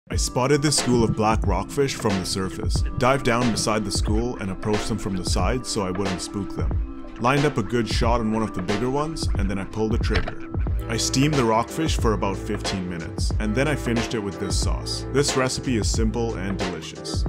Steamed Black Rockfish Catch and Cook (SPEARFISHING)
This has got to be one of my favourite ways to cook black rockfish. Its so fresh and the meat has a beautiful texture. This recipe is super easy and delicious.